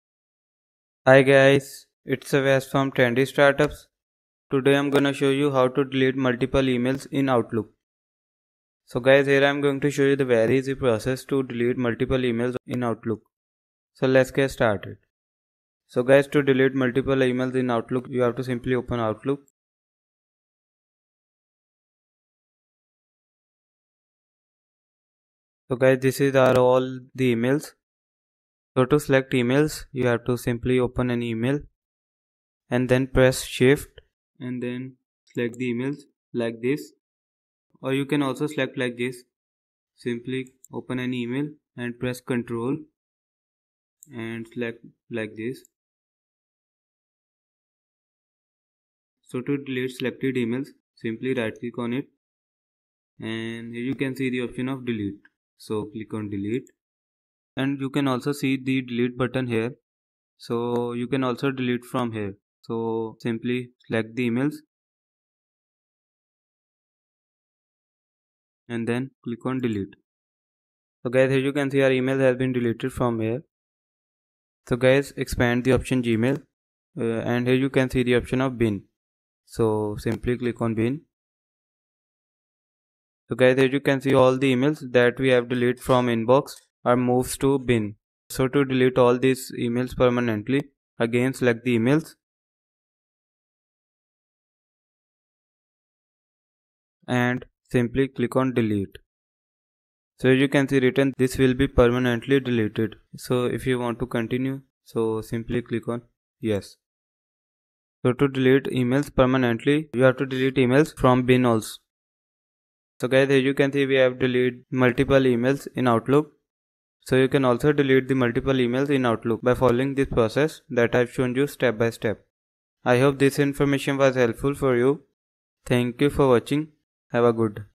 ✅ How to delete multiple emails in Outlook
Solved - How to delete multiple emails in Outlook

Hi Guys, In this video I will show you How to delete multiple emails in Outlook.

I hope, This video will help you. So keep watching this video to delete multiple emails in Outlook and if you want more of these little tips and tricks, then please do subscribe to our channel and give a thumbs up.